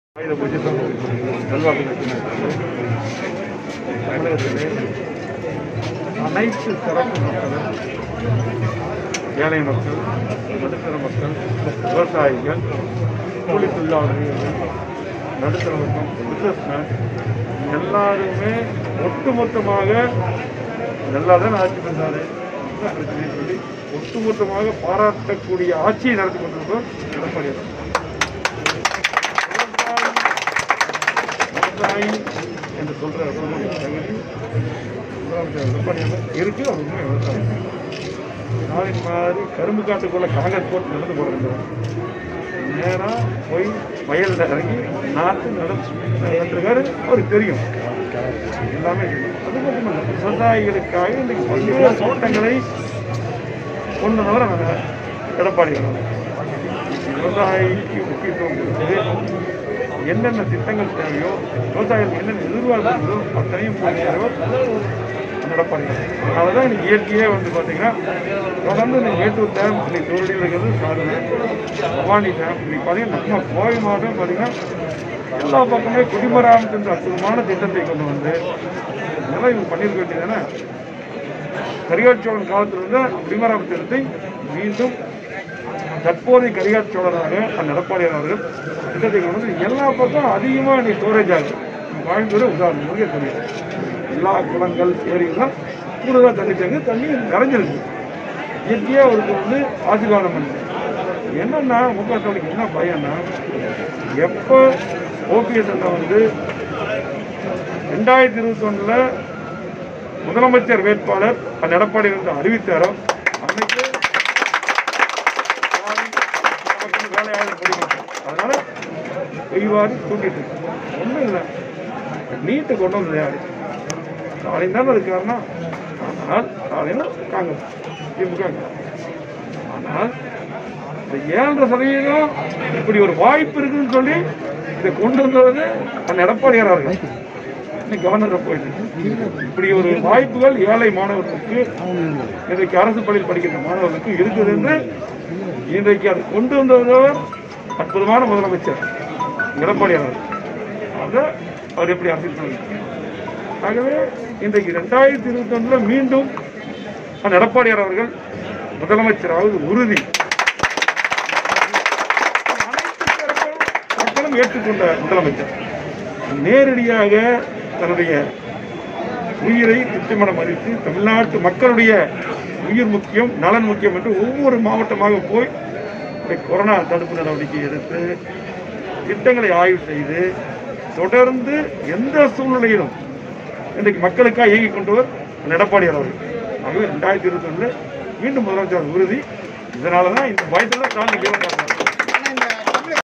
|விவசாயி என்று சொல்லுகின்ற தகுதி முதல்வர் பழனிச்சாமிக்கு தான் உள்ளது| |அமைச்சர் எஸ்.பி.வேலுமணி|
கோவை மாவட்ட அதிமுக தலைமை அலுவலகமான இதயதெய்வம் மாளிகையில் ஆயுத பூஜை விழா நடைபெற்றது இதில் உள்ளாட்சித் துறை அமைச்சர் எஸ் பி வேலுமணி கலந்துகொண்டு ஆயுத பூஜையை கொண்டாடினார் அதனை தொடர்ந்து செய்தியாளர்களை சந்தித்த அவர் பேசுகையில்., தமிழக முதல்வர் எடப்பாடியார் அற்புதமான ஆட்சியை நடத்திக் கொண்டிருக்கிறார். முதலில் உங்கள் அனைவருக்கும் விஜயதசமி ஆயுத பூஜை மற்றும் சரஸ்வதி பூஜை நல்வாழ்த்துக்கள் இன்றைய தமிழகத்தில் உள்ள அனைத்து தரப்பு மக்களும் குறிப்பாக ஏழை நடுத்தர மற்றும் விவசாயக் கூலித் தொழிலாளர்கள் மற்றும் தொழிலாளர்கள் என ஒட்டுமொத்தமாக அனைவரும் பயன்பெறும் வகையில் நன்றாக ஆட்சி செய்து கொண்டுள்ளார் பாராட்டக்கூடிய ஆட்சியை எடப்பாடியார் நடத்திக் கொண்டிருக்கிறார் விவசாயி என்று சொல்லுகின்ற தகுதி முதல்வர் எடப்பாடி அவர்களுக்கு உள்ளது  அவர் உண்மையான விவசாயி ஸ்டாலின் போல் கரும்புக் காட்டுக்குள் கான்கிரீட் போட்டு நடந்து செல்வது இல்லை நடிப்புக்காக விவசாயி போல் வேடம் ஏற்கிறார் விவசாயிகளுக்காக பல்வேறு திட்டங்களை கொண்டு வந்தவர் அண்ணன் எடப்பாடி யார் என்று விவசாயிகளுக்கு முக்கியத்துவம் கொடுத்து என்னென்ன திட்டங்கள் தேவையோ மேலும் விவசாயிகளின் எதிர்பார்ப்புகள் என்ன என்பதை அறிந்து பூர்த்தி செய்தவர் அண்ணன் எடப்பாடி அவர்கள் இன்றைக்கு மேட்டூர் அணையில் 100 அடி தண்ணீர் உள்ளது கோவையில் பவானி அணை மேலும் குடிமராமத்து என்ற அற்புதமான திட்டத்தை கொண்டு வந்தது என இவற்றை எல்லாம் இவர்கள் செய்து இருக்க வேண்டியதுதானே ஆனால் கரிகாற் சோழன் காலத்தில் இருந்த குடிமராமத்து திட்டத்தை மீண்டும் கொண்டு வந்தவர் எடப்பாடியார் தற்போதைய கரிகாற்சோழன் ஆக திட்டத்தை கொண்டு வந்தவர் அண்ணன் எடப்பாடி அவர்கள் எல்லா ஏரிகள் மற்றும் குளங்களையும் கூடுதலாக தண்ணீர் தேங்கியுள்ளது நிறைந்துள்ளது இயற்கையை அவருக்கு ஆசீர்வாதம் செய்கிறது
முக ஸ்டாலினுக்கு என்ன பயம் என்றால் எப்போது ஓபிஎஸ் அவர்கள் 2021ல் முதலமைச்சர் வேட்பாளர் அண்ணன் எடப்பாடி யார் என்று அறிவித்தார் அன்றைக்கே ஸ்டாலின் நமக்கு வேலை ஆகாது என்று முடிவு செய்து விட்டார் அதனால் தான் பொய்களை வாரி தூற்றி கொண்டுள்ளார் நீட் தேர்வை கொண்டுவந்தது ஸ்டாலின் தான் திமுக காங்கிரஸ் ஆர்வம் கொண்டு வந்தனர், ஆனால் 7.5 சதவீத வாய்ப்பு உள்ளது என்பது கொண்டுவந்தது அண்ணன் எடப்பாடி அவர்கள் தான் இன்றைக்கு ஆளுநர் இடமும் சென்று உள்ளது இப்படி ஒரு வாய்ப்பு கள் ஏழை மாணவர்களுக்கும் அரசுப் பள்ளியில் படிக்கின்ற மாணவர்களுக்கும் இருக்கின்றது என்பதை கொண்டு வந்தவர் அற்புதமான முதல்வர் எடப்பாடி யார் தான் இன்றைக்கு 2021 மீண்டும் எடப்பாடியார் முதல்வராவது உறுதி இன்றைக்கு அனைத்து தரப்பு மக்களும் ஏற்றுக் கொண்ட முதல்வர் நேரடியாக தன்னுயிரை துச்சமென மதித்து மக்களுடைய உயிர் முக்கியம் முக்கியம் என்று ஒவ்வொரு மாவட்டமாக சென்று covid-19 தடுப்பு நடவடிக்கை திட்டங்களை ஆய்வு செய்து எந்த சூழ்நிலையிலும் மக்களுக்காக இயங்கிக்கொண்டு உள்ளவர் அண்ணன் எடப்பாடி யார் 2021 மீண்டும் முதல்வராவது உறுதி இந்த பயத்தில்தான் ஸ்டாலின் பேசி உள்ளார் என்றார்.